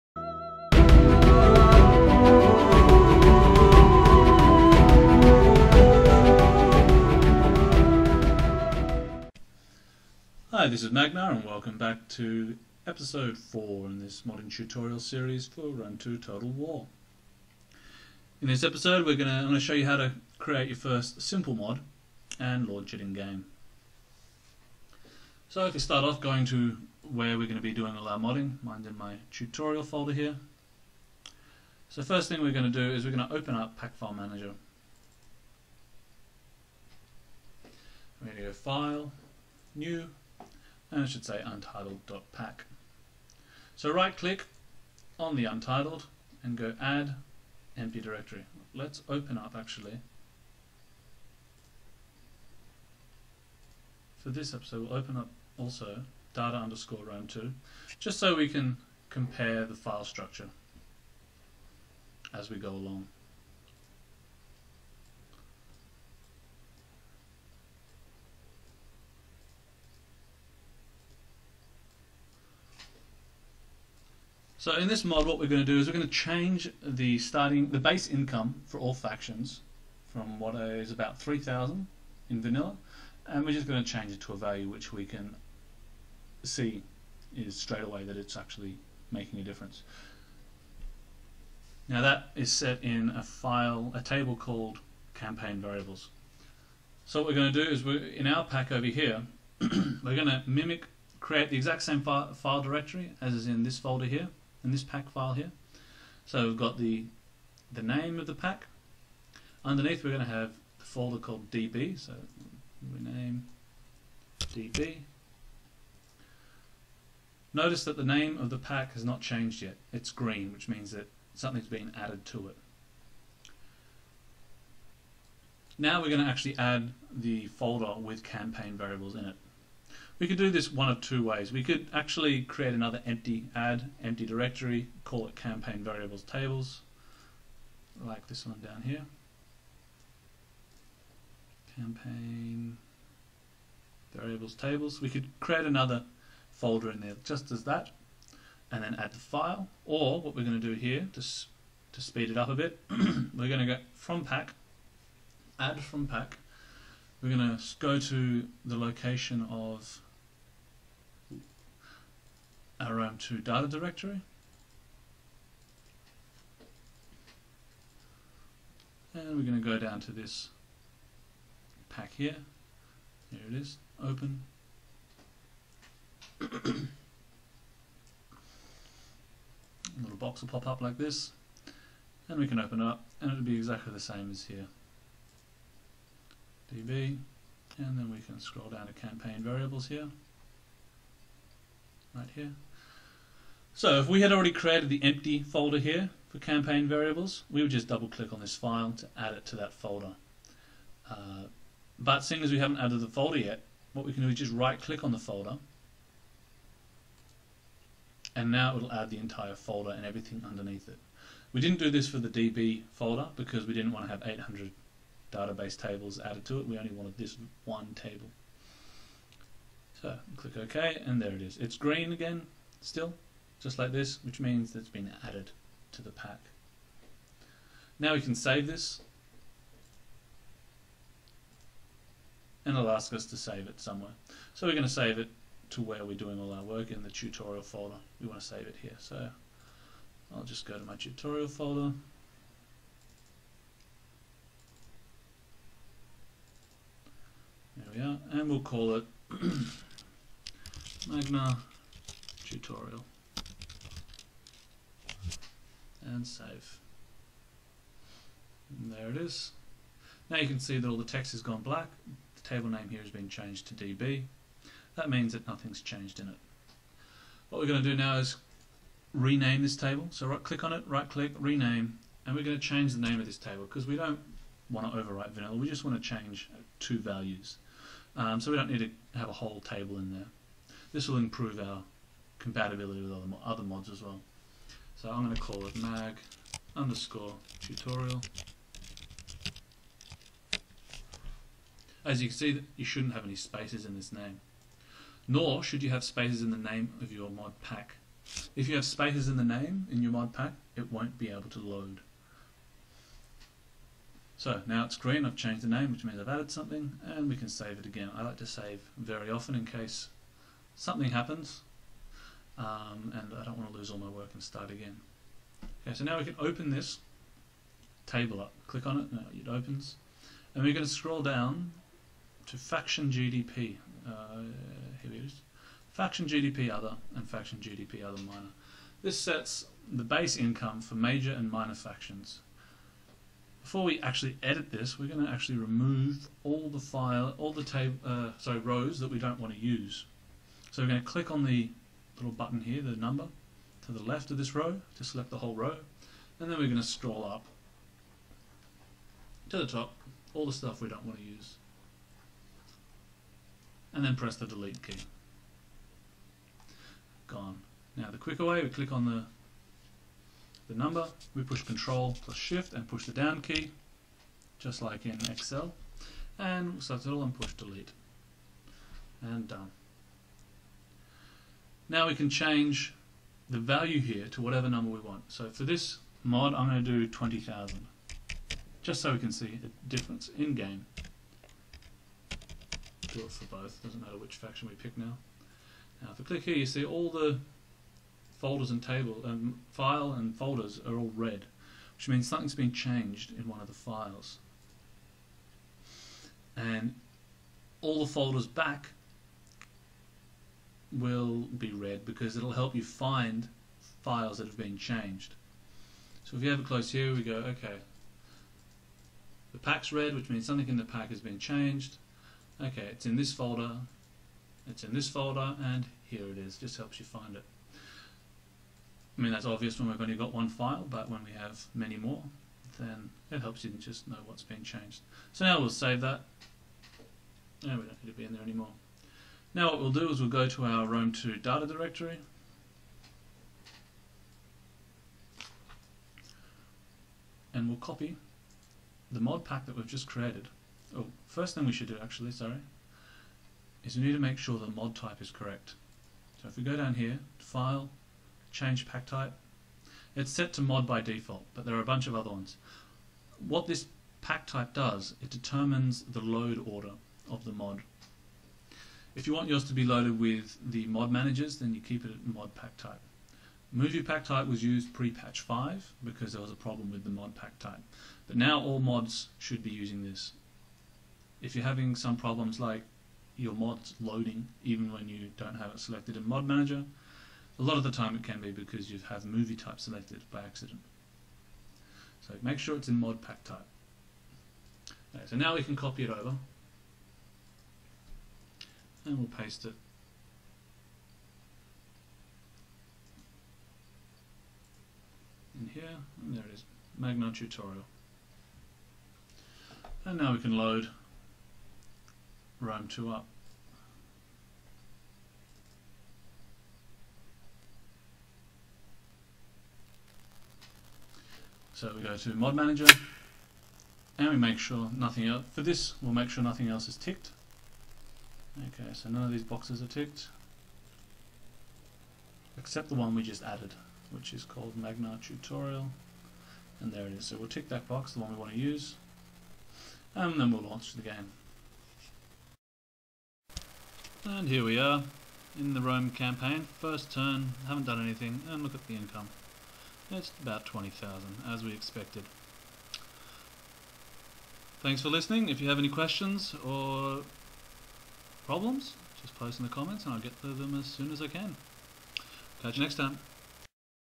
Magnar Tutorial - Episode IV(4) - Creating Simple Mod
Creating a Simple Mod
-Step by step introduction on how to make a simple mod from scratch.